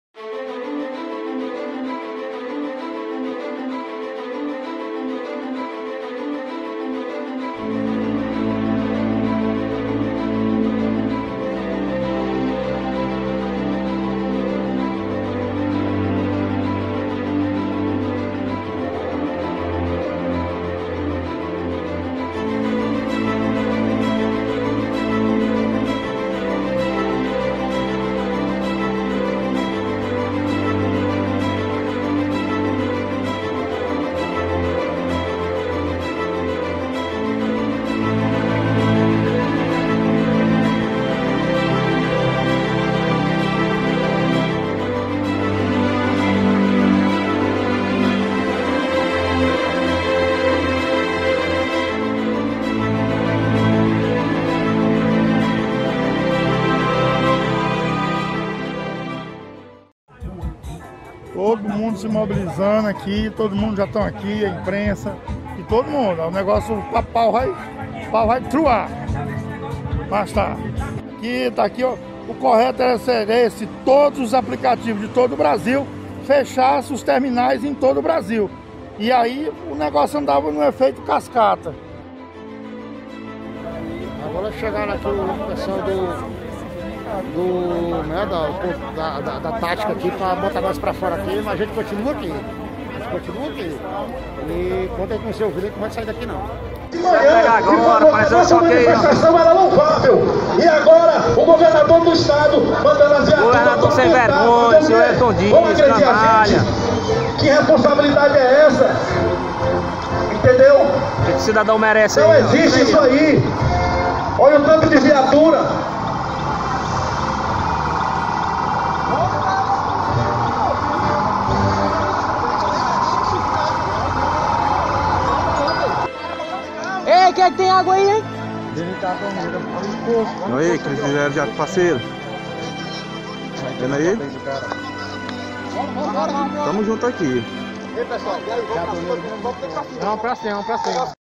Cenas fortes. Confronto entre manifestantes e a polícia de Wellington Dias
Motoristas de aplicativos fizeram manifestação nesta quarta-feira (10) em frente ao Terminal de Petróleo. O manifestante Thiago Cordeiro contou que os motoristas não aguentam mais os aumentos constantes no preço da gasolina. Eles bloquearam a passagem de caminhões. Ninguém entrou ou saiu por várias horas. Houve determinação do governo Wellington Dias (PT) para que a polícia militar desocupasse a área.

Houve confronto e alguns feridos. Motoristas exibiram marcas do que seriam balas de borracha utilizadas por policiais. Os motoristas disseram ter sido alvo de violência.  "Fomos vítimas de repressão policial", comentou um deles. A polícia não se manifestou sobre a denúncia. Os vídeos sobre o protesto e o confronto circularam nos grupos de WhatsApp ao longo da noite e madrugada.

Em Teresina/PI, o litro da gasolina já passa dos R$ 5,55 e do diesel R$ 4,49. Um novo aumento deve ocorrer na próxima terça-feira(16). Houve protestos em diversas cidades do país pedindo a reabertura das atividades comerciais. O presidente do sindicato de postos, Alexandre Valença, atribui o alto preço no Piauí ao ICMS cobrado pelo governo.